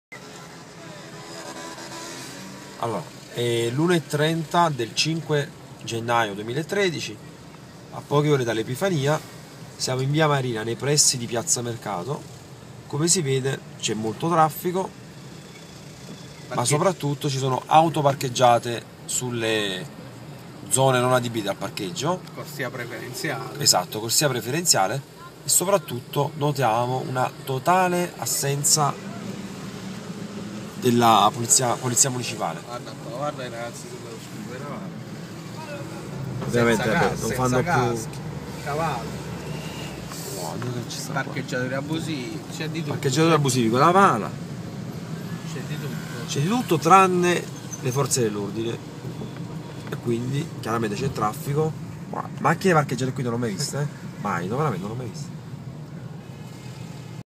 Napolinferno - la befana
Rettifilo e via Marina paralizzate, parcheggiatori abusivi ovunque e auto parcheggiate persino sui mini marciapiedi della corsia preferenziale a ridosso di piazza Mercato. Sindaco, dove sei???